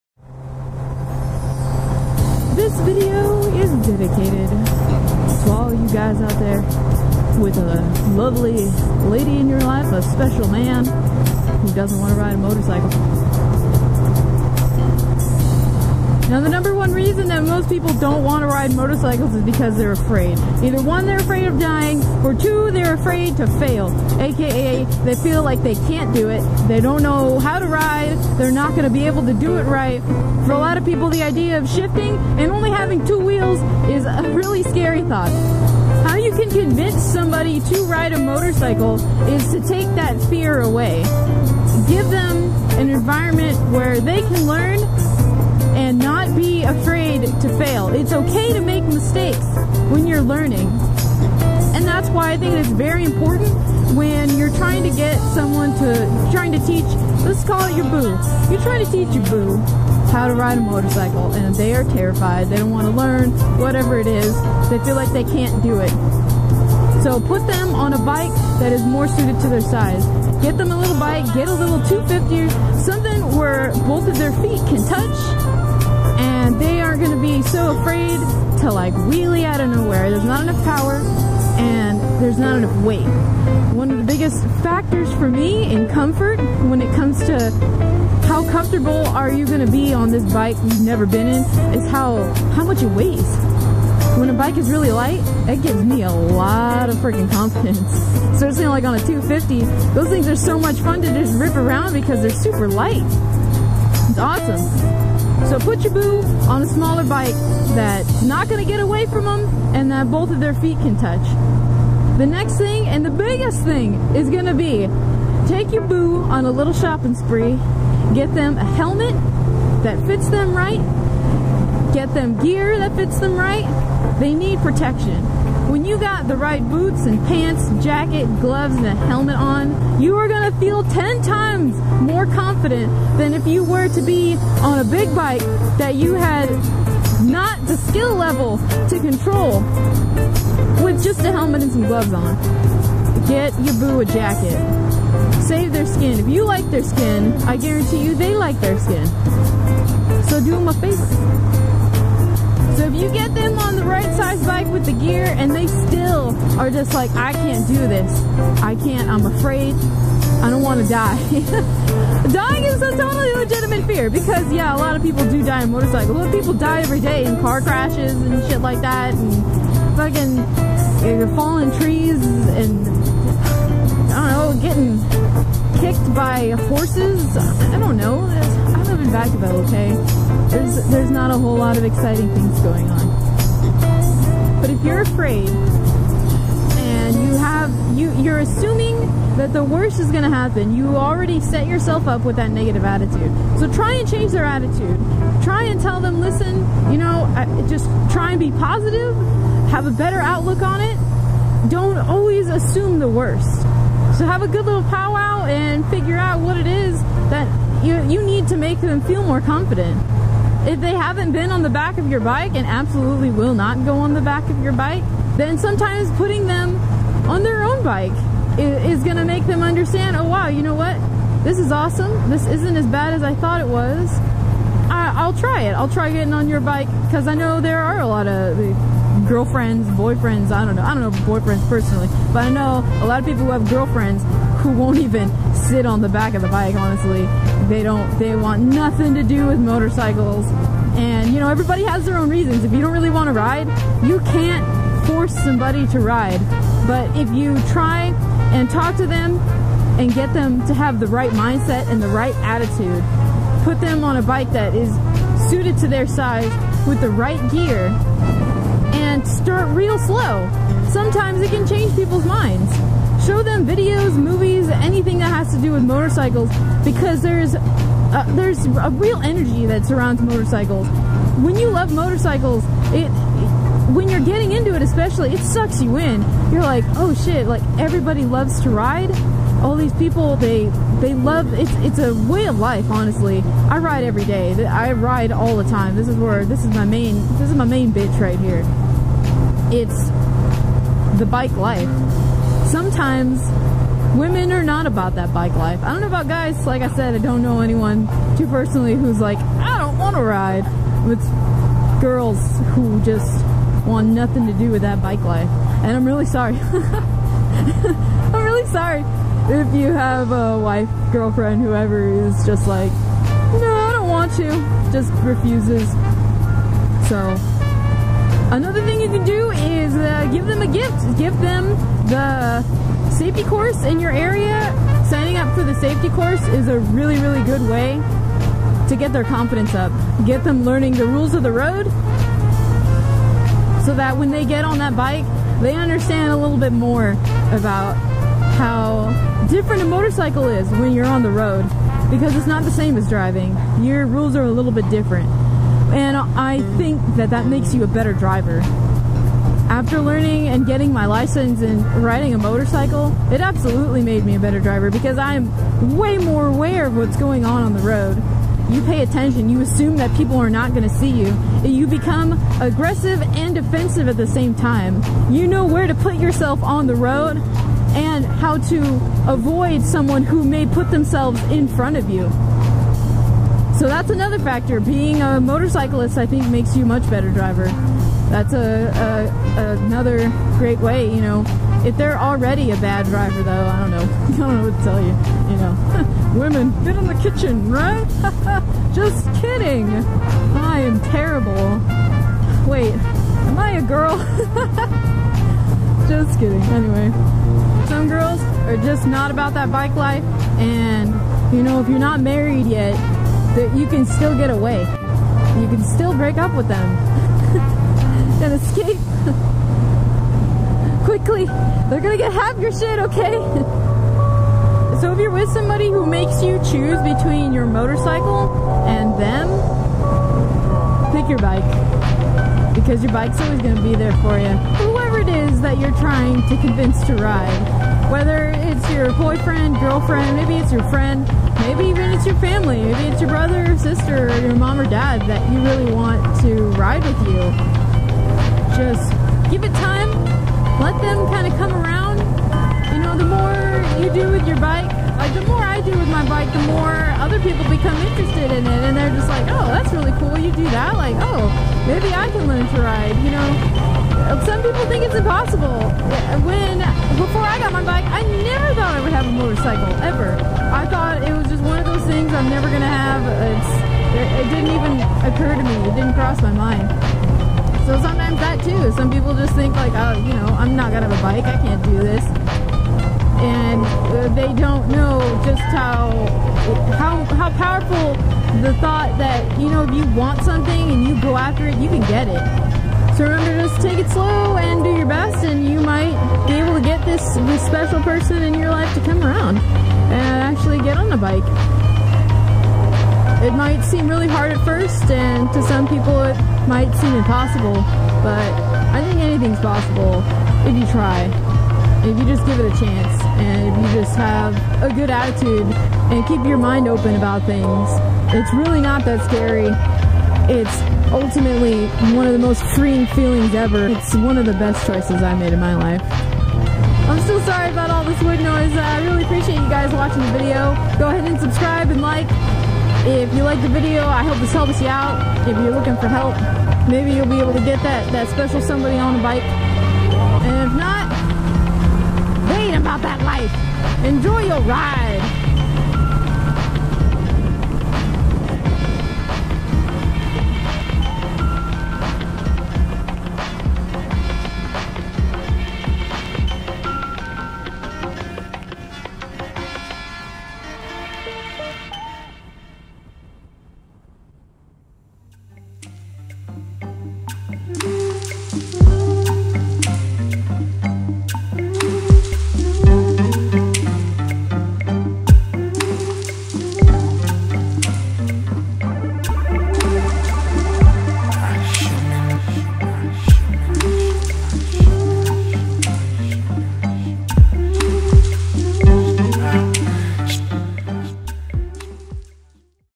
How to Convince Your Girlfriend to Ride A Motorcycle
I give my tips on how you can convince your significant other to be as enthused about bikes as you are.

Sorry if the audio isn't the best, it's been windy out here.